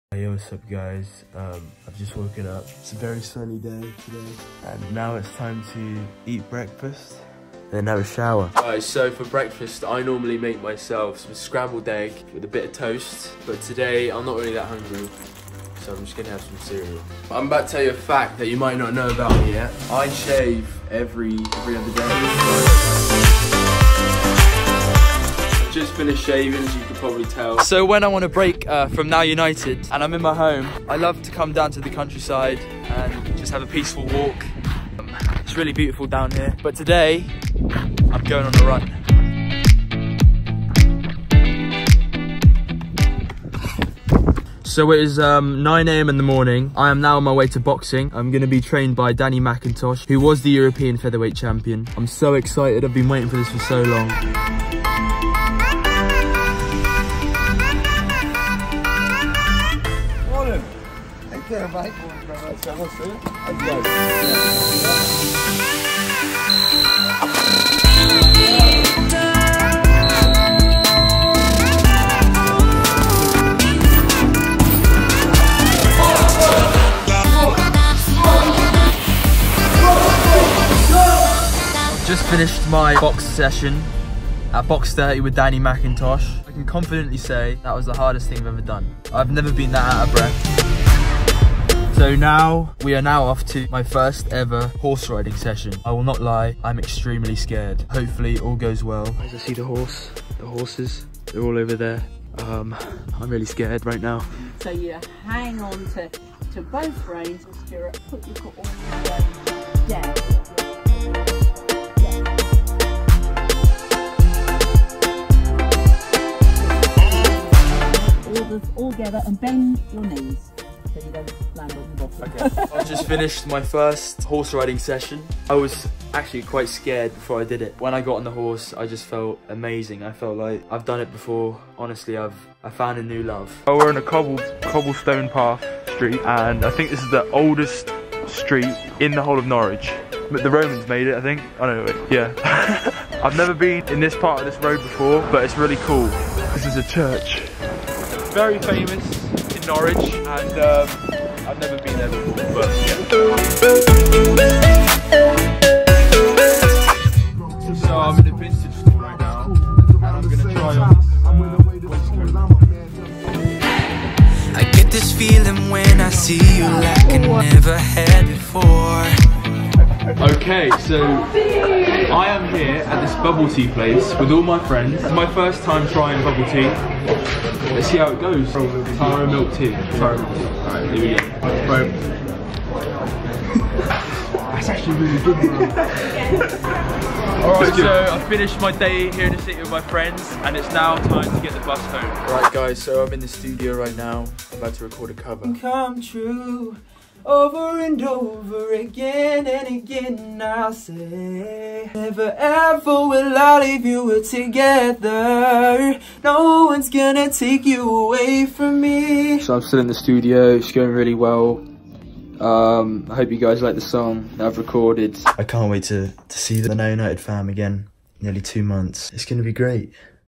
Horses, Boxing, and Boba with Bailey - This Is Me
Bailey starts his week off by preparing breakfast and showing his morning routine. He then takes a boxing class and learns to ride a horse! Later, Bailey heads to the city to shop, drink boba and hang with friends. He ends the week recording a song. Hope you like it!